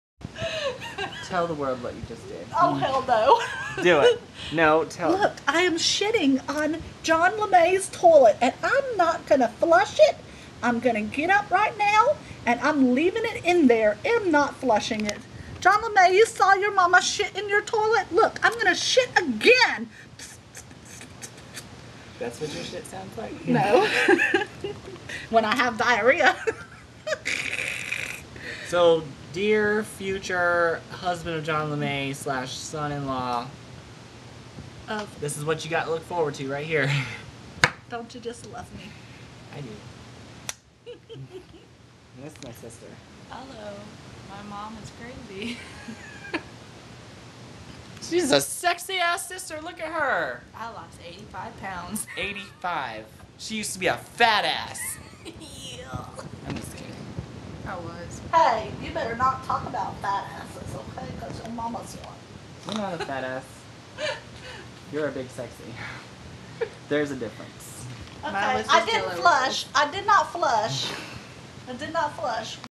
my mom loves facebook
she loves playing yoville. this is my mom and sister!